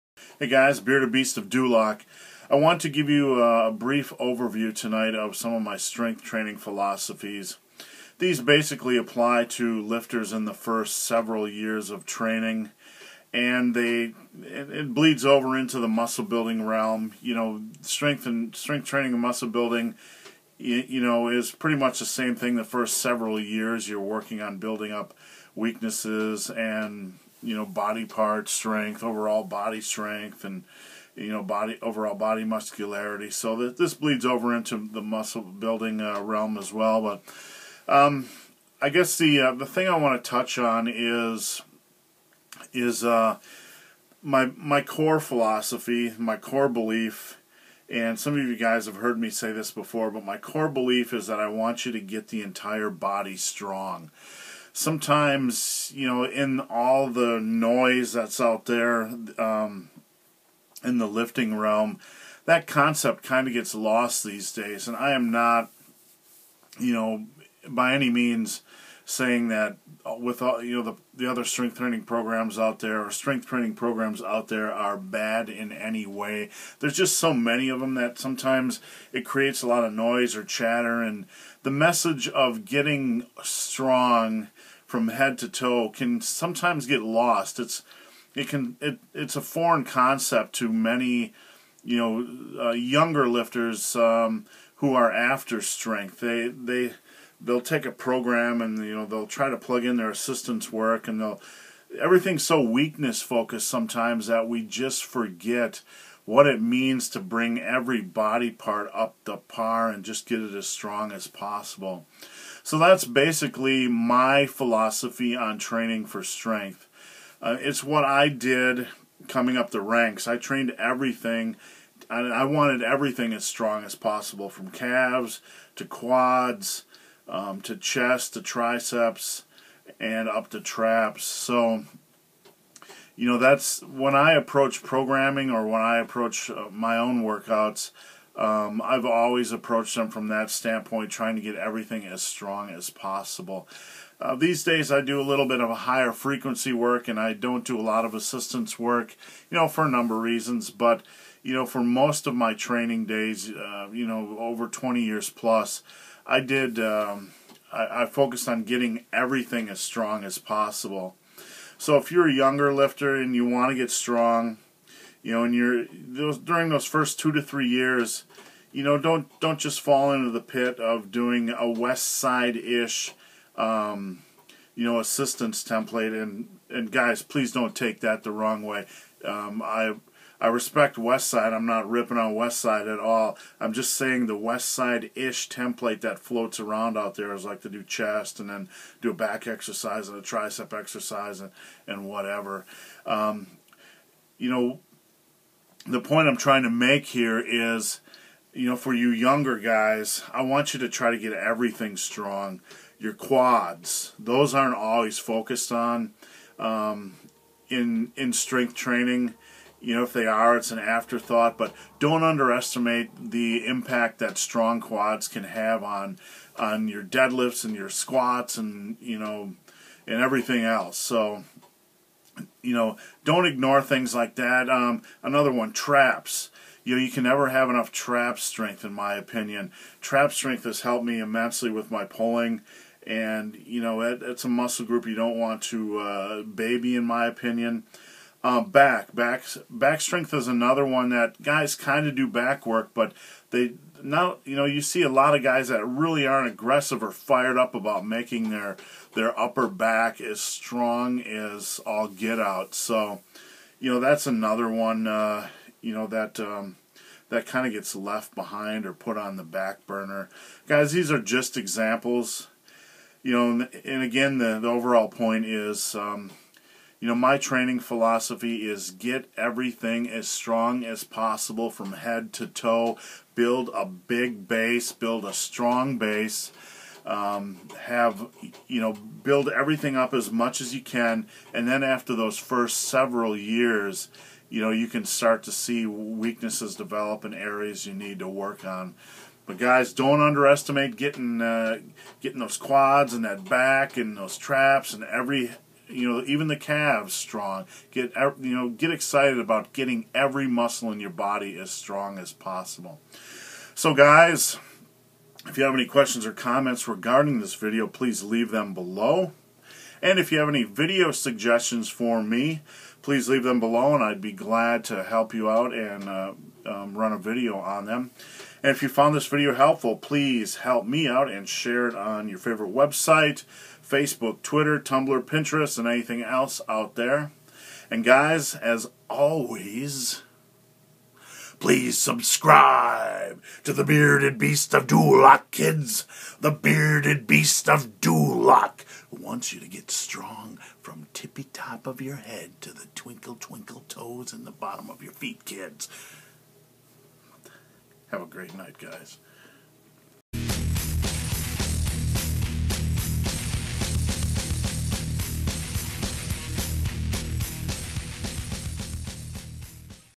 My Strength Building Philosophy; Get Strong - BBOD #45
My philosophy is simple: get strong from head to toe. Learn how I recommend that you train during your first several years.